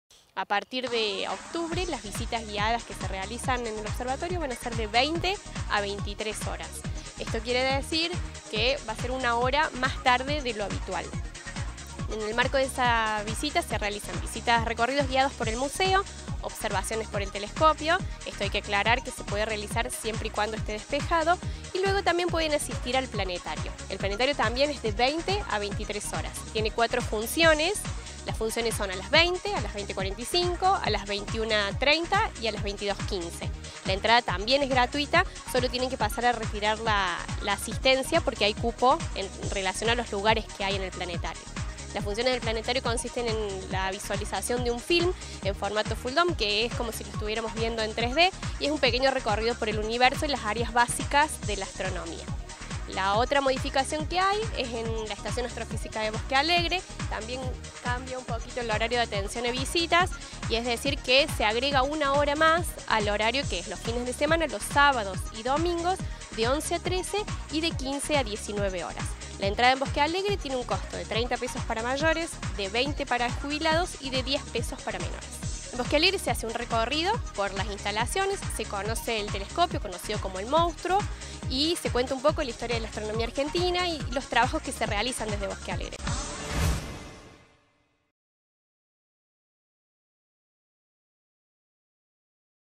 visitas en el Observatorio Astronómico de Córdoba - 10.10.17
A partir de Octubre y hasta Marzo cambian los horarios de visitas en el Observatorio Astronómico de Córdoba y en la Estación Astrofísica de Bosque Alegre.